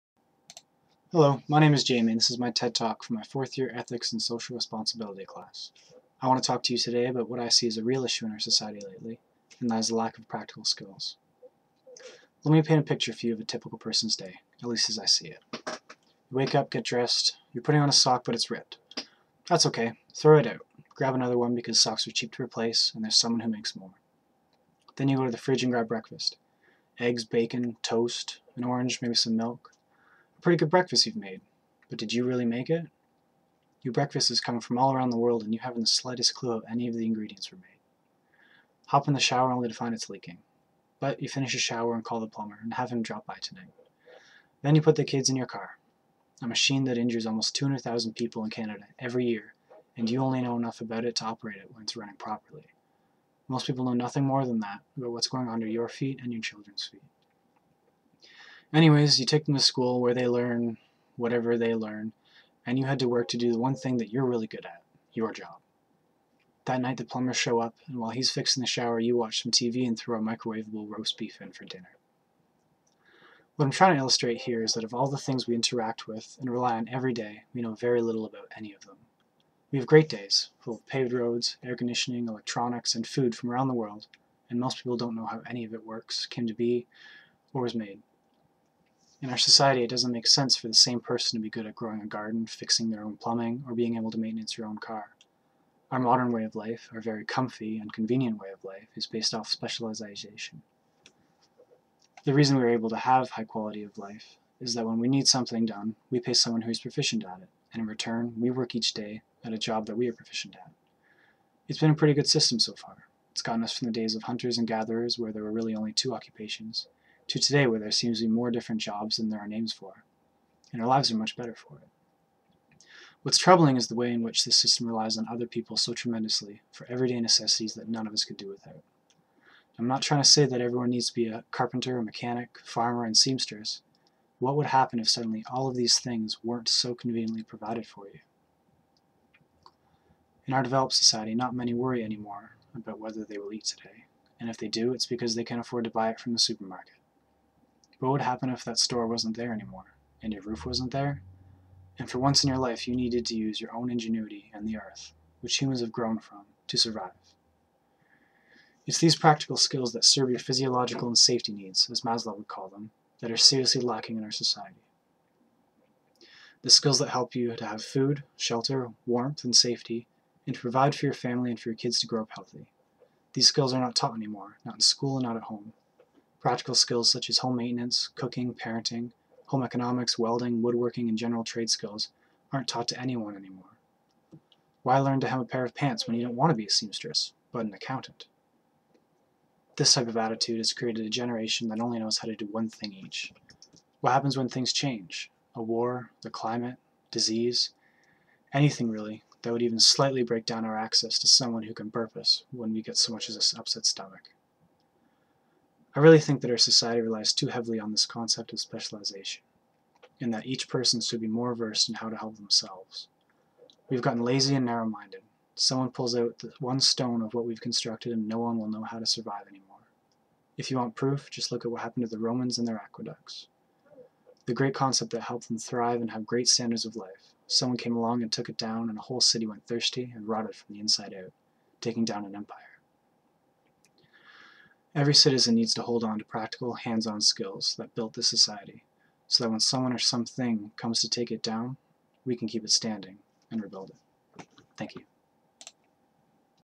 ENG 4A03 TED Talk, James Ververs
This is a TED-type talk made for my ethics and social responsibility class assignment.